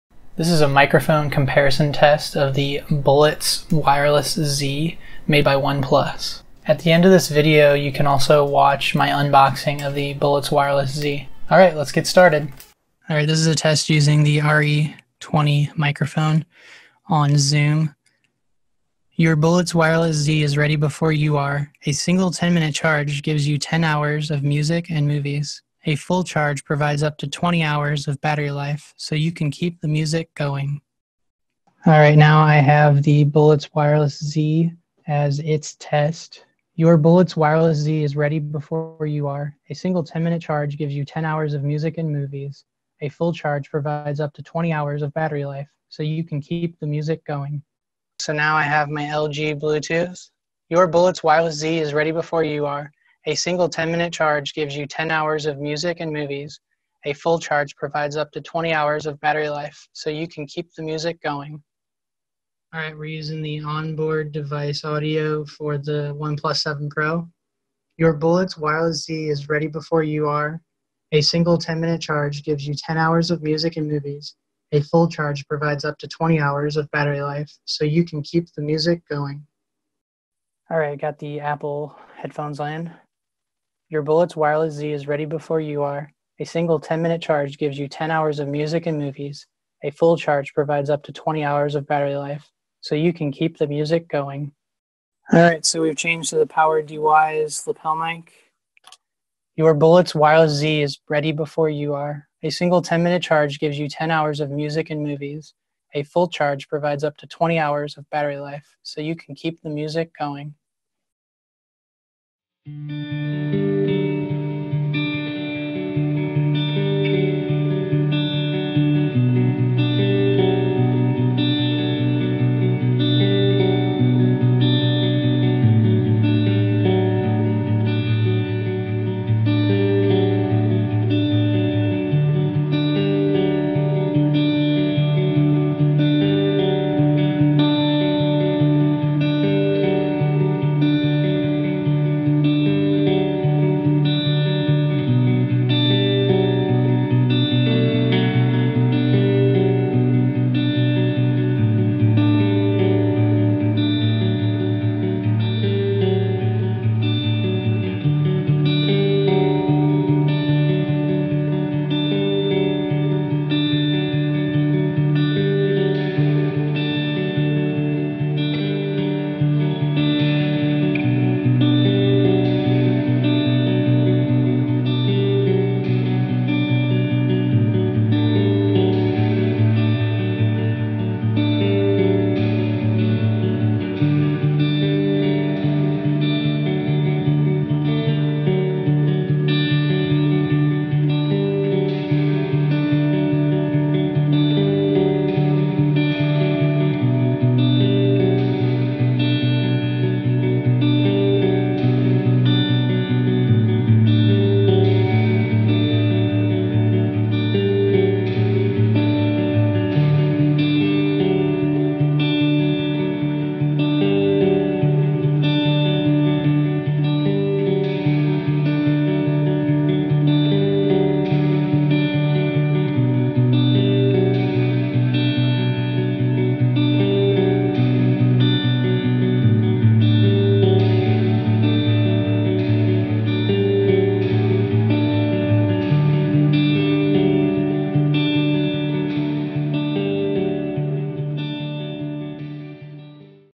OnePlus Bullets Wireless Z: Mic Comparison Test
I recorded all of the tested audio from a recorded Zoom call. The audio was only amplified in Audacity. All sound recordings were accomplished in a sound proofed studio. The Bullets Wireless Z came with several different sized ear pieces.